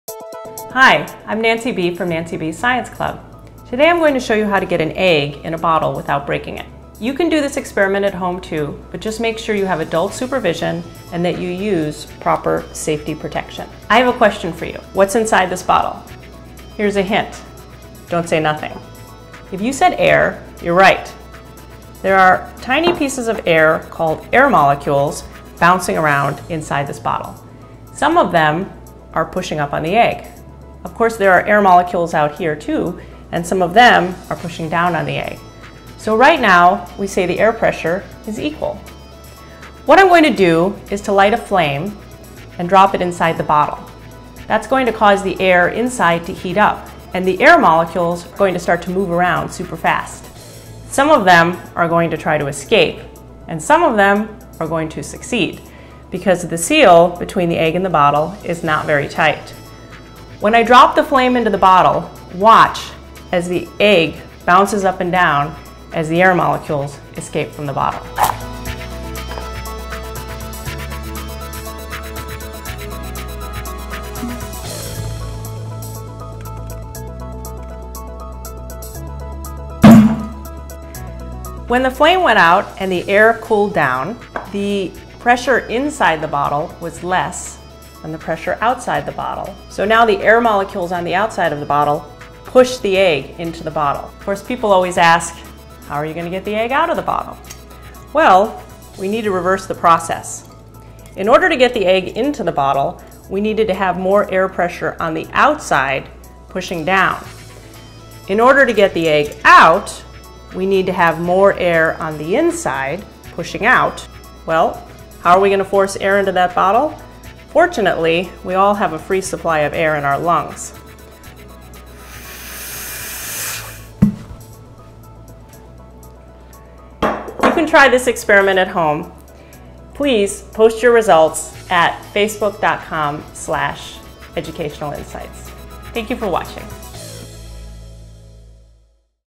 Egg-stra Special Demonstration: Egg in a Bottle Science Experiment by Nancy B's Science Club
Are you ready for an egg-cellent demonstration about the workings of air pressure? Take a crack at our egg in a bottle science experiment to learn more!

Video Transcript: Hi, I'm Nancy B from Nancy B’s Science Club. Today  I'm going to show you how to get an Egg in a Bottle without breaking it. Using a few items right at home, you can do this experiment too. But make sure to use safety equipment, and invite an adult to help you.

Here’s what you’ll need:

A Wide-mouth bottle (or flask)
Hardboiled, pre-peeled Eggs (MEDIUM sized –not L or XL)
Wooden Stirring Rods
Laboratory glasses
And most importantly, Adult Supervision

Here’s a question for you: what’s inside the bottle?

If you said AIR, you’re right!

There are little pieces of air—we call them air molecules—bouncing all over the place and pushing on things. Some of them are pushing up on the egg.
Of course, there are some air molecules on the outside pressing down on the egg.

In fact, the air pressure in both directions is equal.

But if we heat the air inside, the heat is going to make the air molecules move around super fast. Some of them are going to manage to escape right around the rim of the egg because this is not a very tight seal between the egg and the bottle.
When I drop the candle in, watch how the egg bounces up and down as the air escapes.

After the air escapes from inside the bottle and the flame goes out, the air inside the bottle cools down and is pushing up less than the air on the outside pushing down. The air molecules on the outside push the egg into the bottle.

So how do you get the egg out without breaking it? We need to reverse the process. To get it IN, we needed more air on the outside than the inside. To get it OUT, we need more air pressure on the inside than the outside; then the extra air pressure on the inside will push the egg out.
Where do we all have a free supply of air? Our lungs!
Watch my cheeks puff up as I blow into this bottle.

That was fun! Make sure that you share your results of your egg in a bottle science experiment